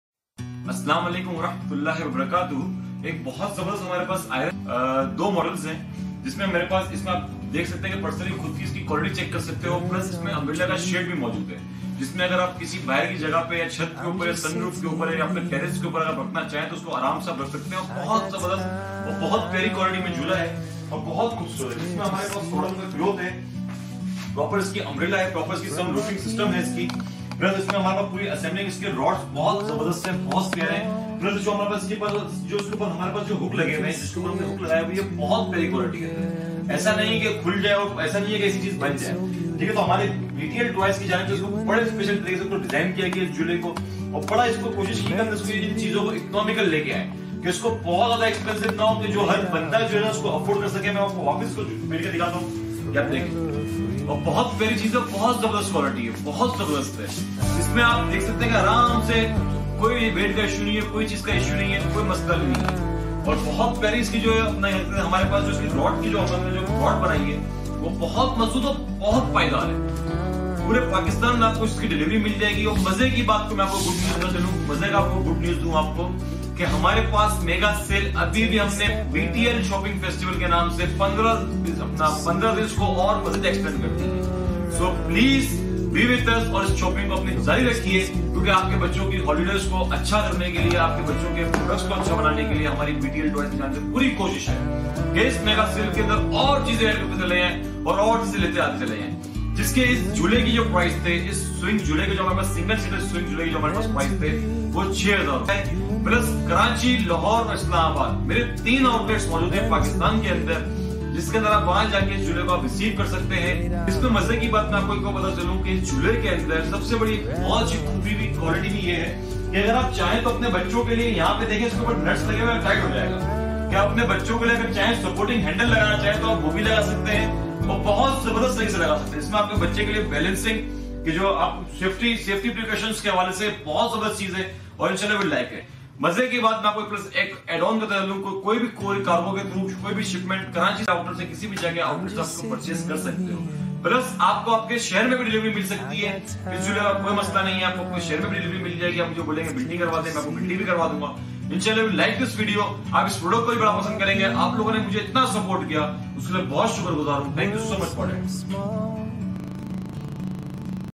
Garden Swing For Kids | Patio Swing with Canopy | Baby Jhula
Garden Swing For Kids | Patio Swing with Canopy | Baby Jhoola
To relax your kids from their daily school routine BTL toys have launched a patio swing jhoola by sitting on this jhoola your kids can relieve tension and enjoy the nature in your garden
You can find different varieties of swing jhoola’s in the market made up of wooden, and plastic that can break easily and cannot carry much weight but BTL TOYS have launched metal swing jhoola that can carry 100 kgs weight want to know more about its unbelievable features so let's go:
Metal Garden swing for kids. With top quality metal rod which supports 100 kg weight.